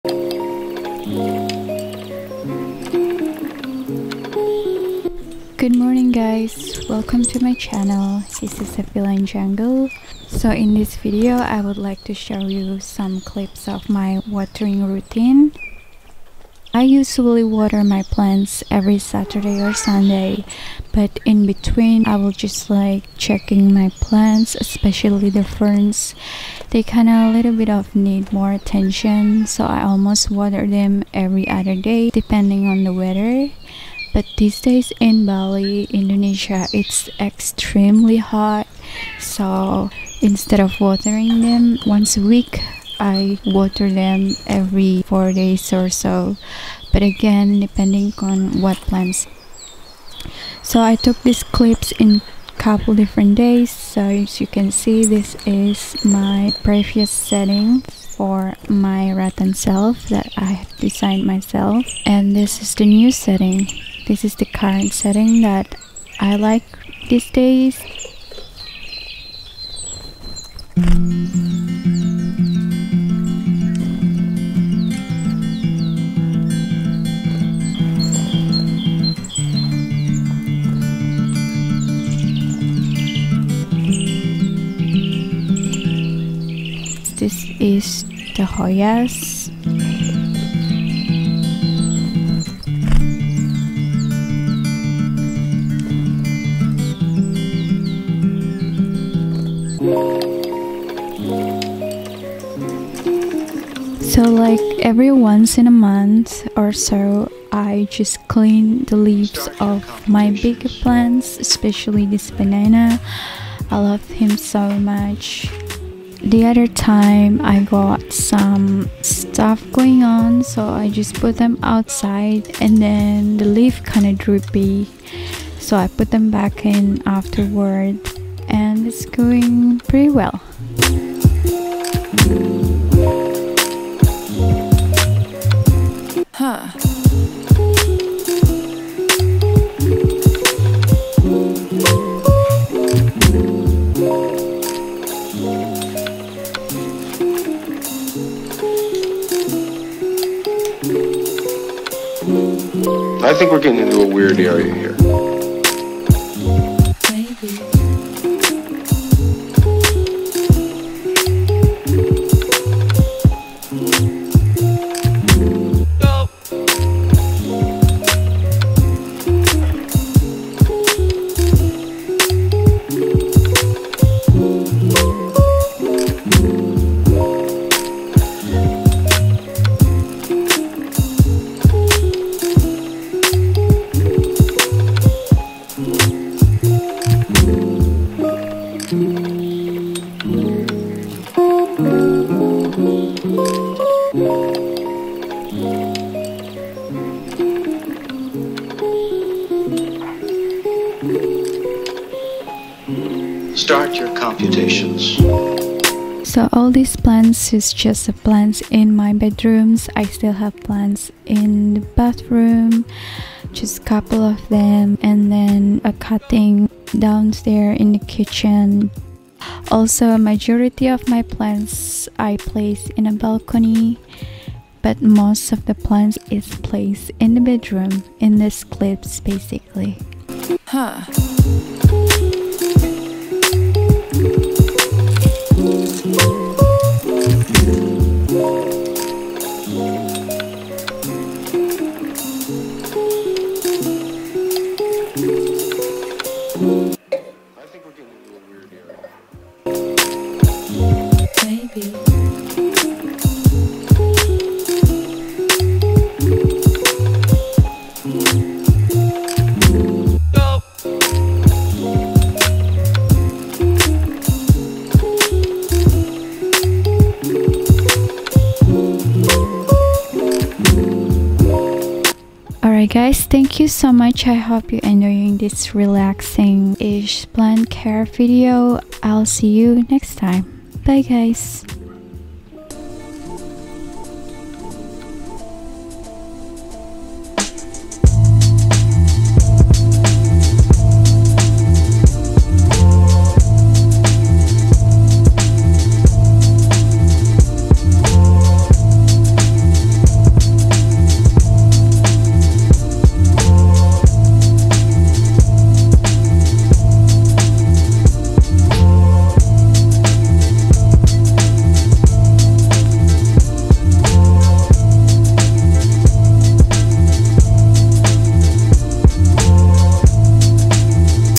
Bedroom weekly Plant Care in 5 minutes [rutinitas menyiram tanaman]
**Klik CC untuk mengaktifkan sub Bahasa Indonesia**

Cameo                     : my rescue cat Maximilian